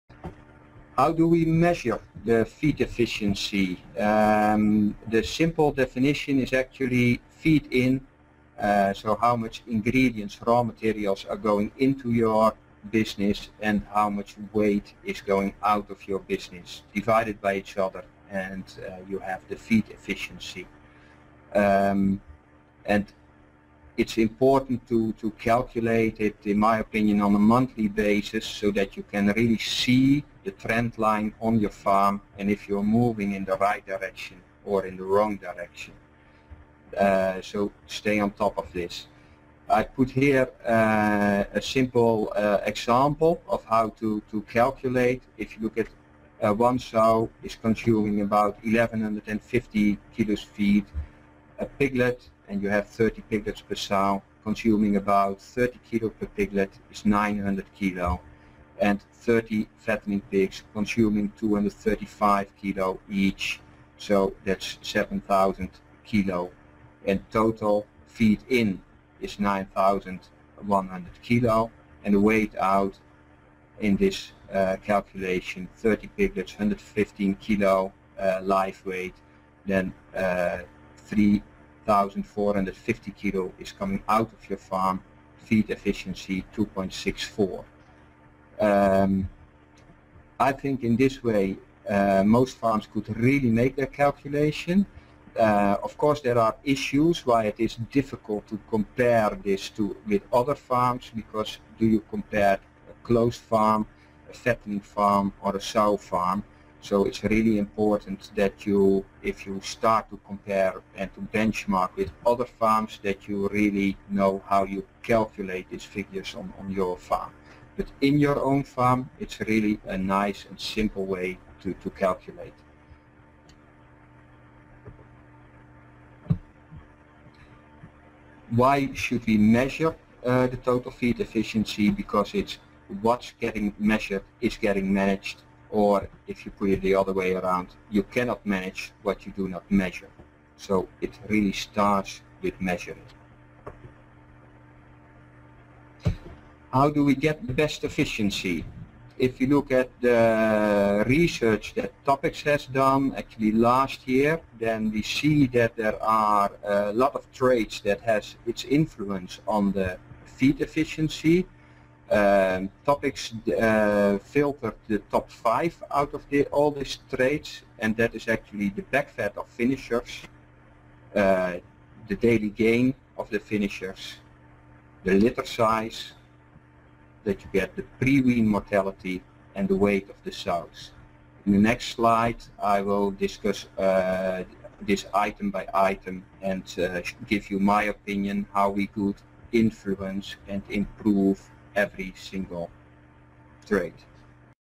Measuring, managing pig feed efficiency
Philip van den Brink, founder of SwiNco, explains how to measure feed efficiency and the importance of measurement. What gets measured gets managed; otherwise you cannot manage what you do not measure. Van den Brink also discusses influential traits that can be used to get the best feed efficiency. Some influential traits include back fat finishers, litter size and weight of sows. The webinar explains how producing pork efficiently is the key for successful pig farming. By breeding for the highest total feed efficiency you can produce more pork with less feed from farrow to finish. Audience learning points include: How genetics can impact the total feed efficiency of breeding goals; the main factors influencing feed efficiency and what you can do to improve your herd performance; and how to profit from rapid and effective genetic progress.